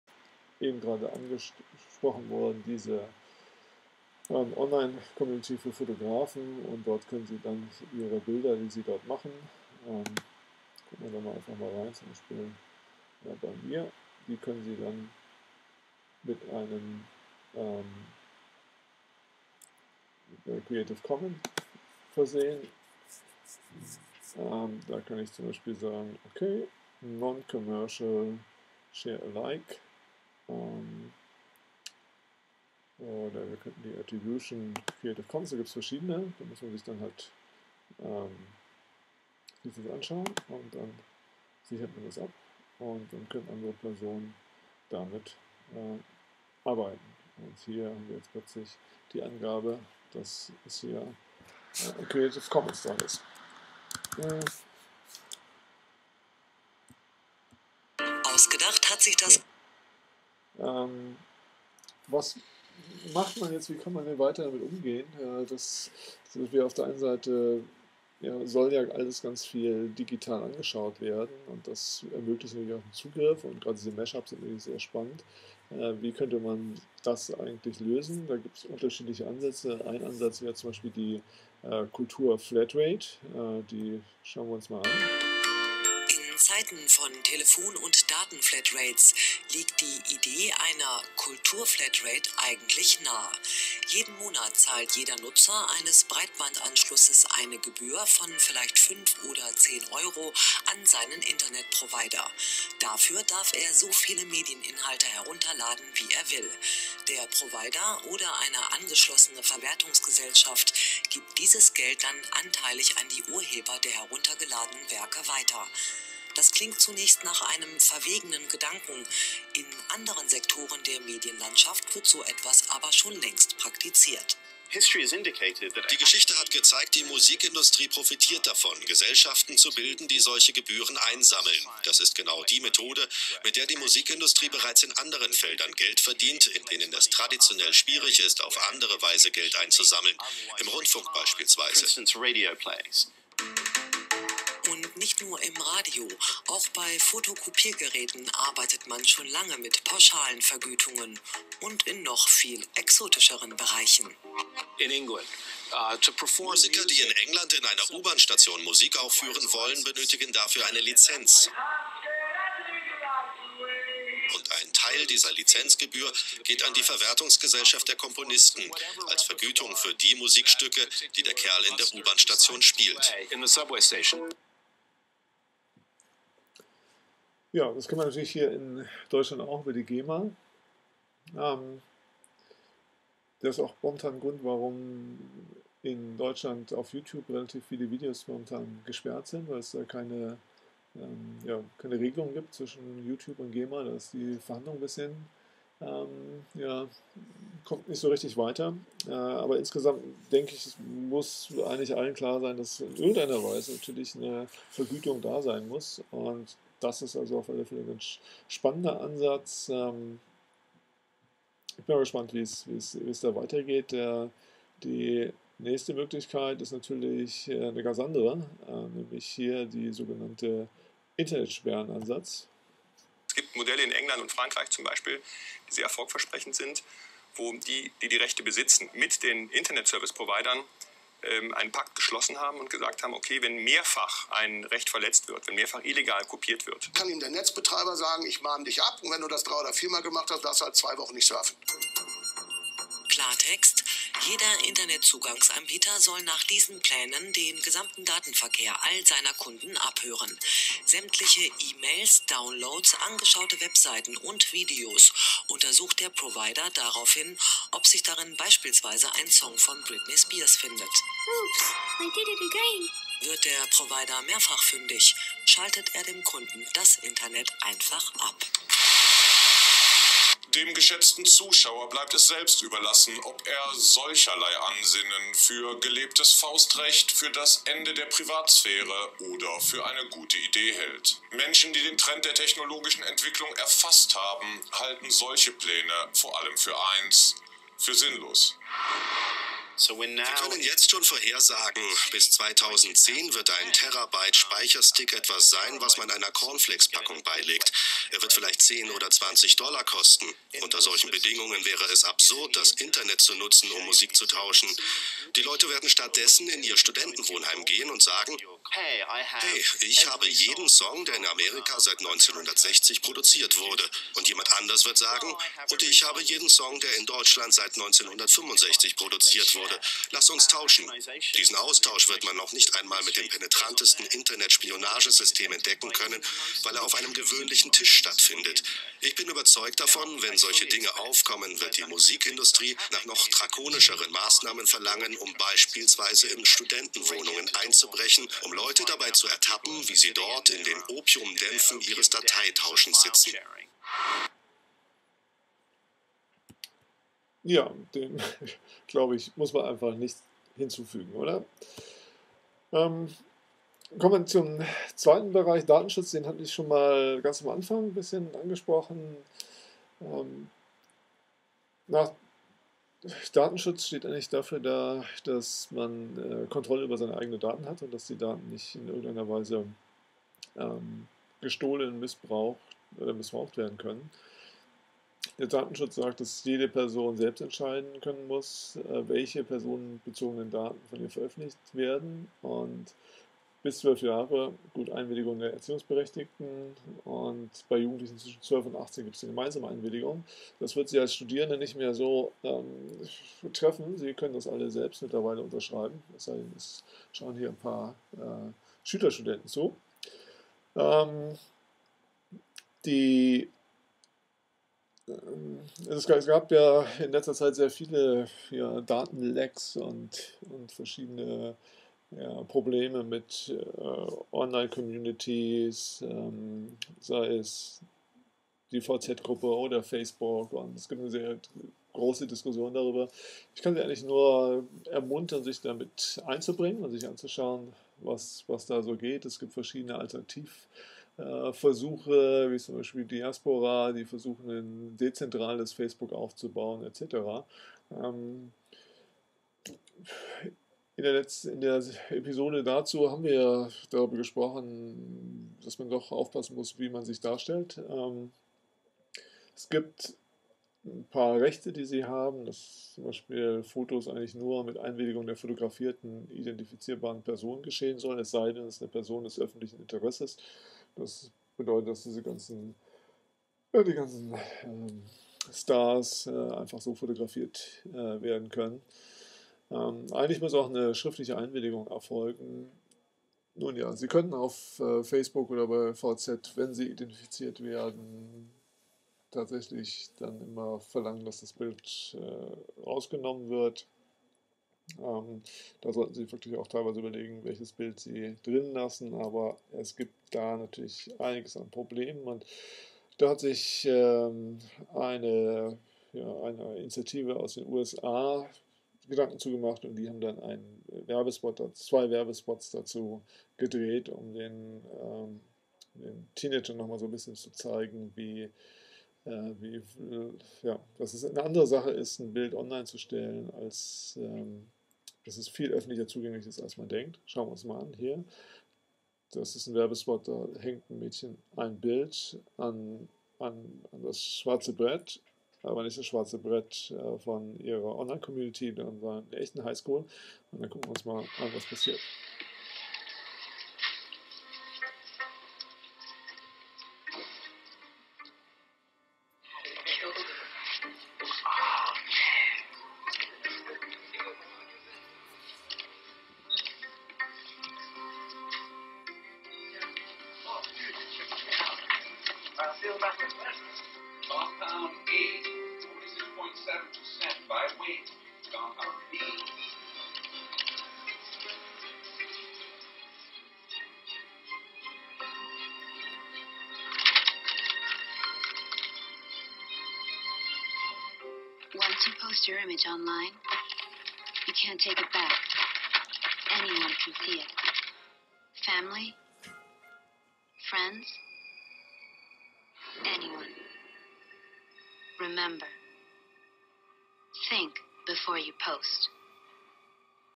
Episode F-03b | Urheberrecht und Datenschutz (Teil 2/3)
Wie mit digitalen Kopien umgehen? Kultur-Flatrate vs. Internet-Sperrung.
Kurzer Hinweis auf Datenschutz (siehe dazu auch unsere Episode zu den sozialen Netzwerken).